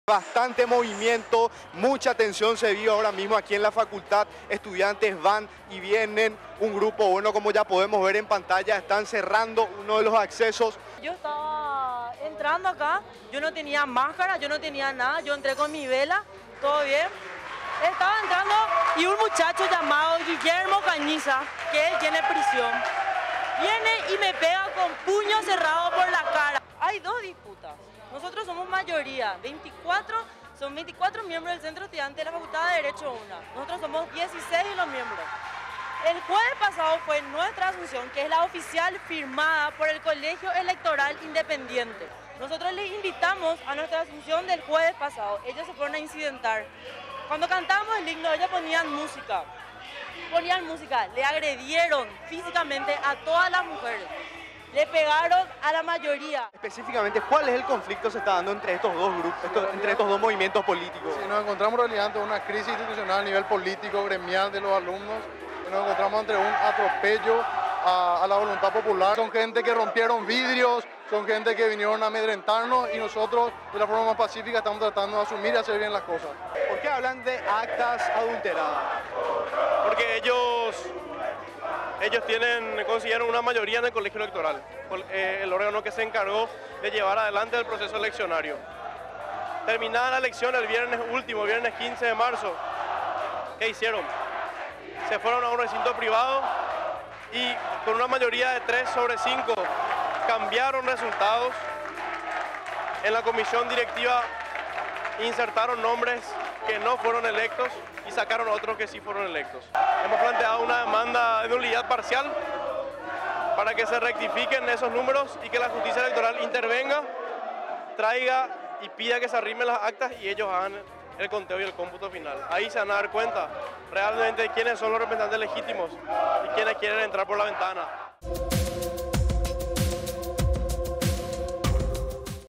Incidentes en Derecho UNA
Enmascarados y con un ataúd ingresaron este martes miembros del movimiento FACU, para impedir la proclamación de las nuevas autoridades del Centro de Estudiantes de Derecho UNA.